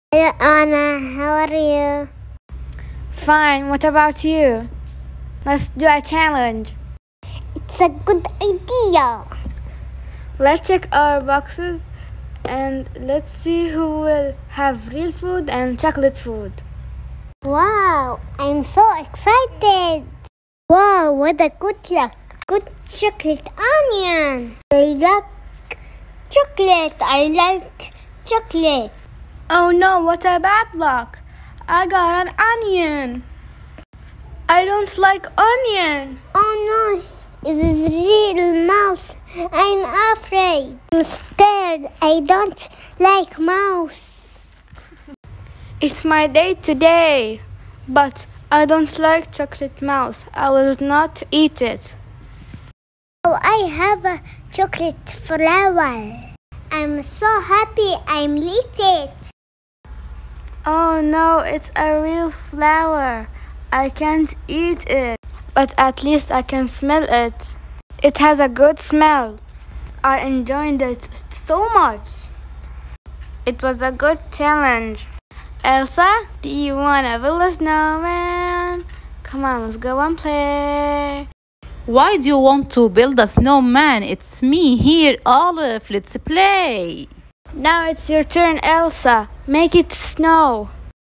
Elsa and Anna do food challenge
Elsa and Anna with real food and chocolate food